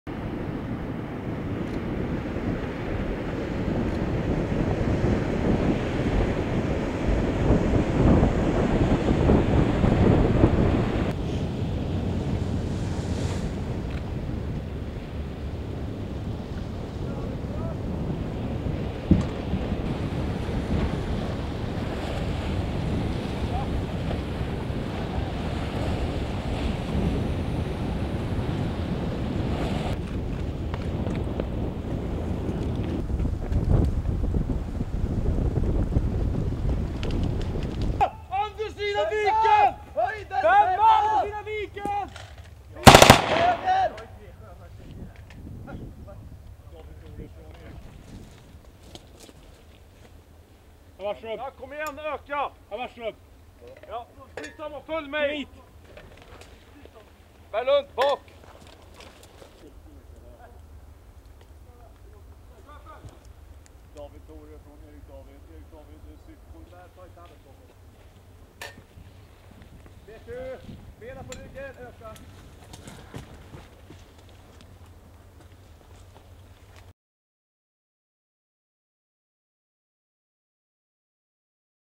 BALTOPS 2016: Swedish Marines Amphib Assault on Uto, Sweden (B-roll)
UTO, Sweden- Swedish Marines participate in an amphibious assault exercise in Uto, Sweden during BALTOPS 2016. BALTOPS is an annual recurring multinational exercise designed to improve interoperability, enhance flexibility, and demonstrate the resolve of allied and partner nations to defend the Baltic region.